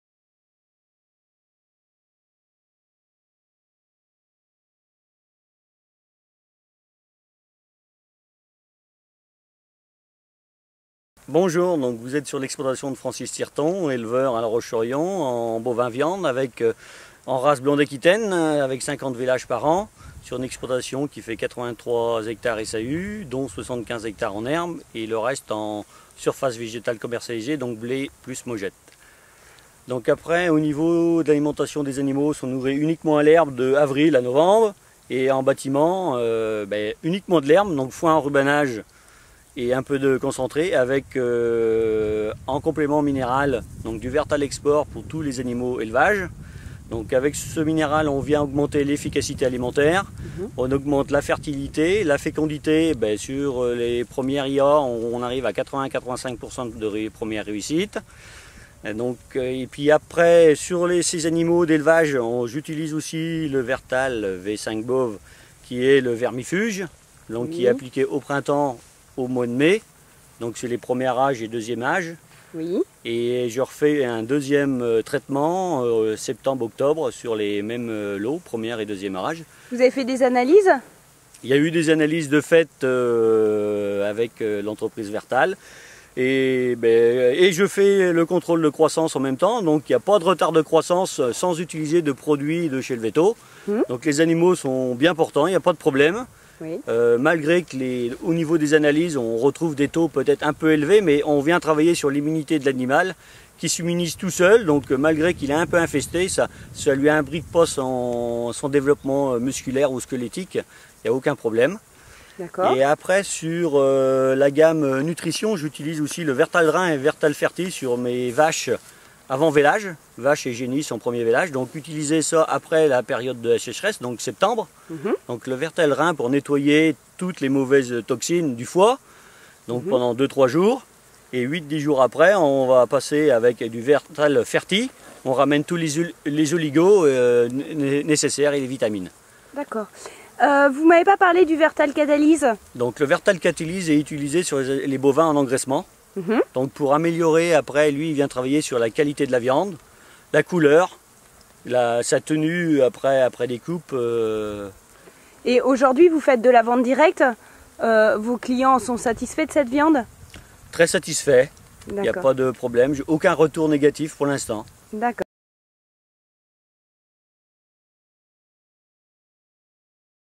Vertal: comment valoriser la ration de vos bovins au quotidien ?
Monsieur Tirton, éleveur vendéen de bovins viande témoigne. Il utilise les solutions Vertal depuis plusieurs années. Il témoigne sur les produits de la gamme Vertal Nutrition animale. Avec l'utilisation du Vertal Export, il a observé des résultats sur la valorisation alimentaire, la fertilité et la fécondité de ses vaches.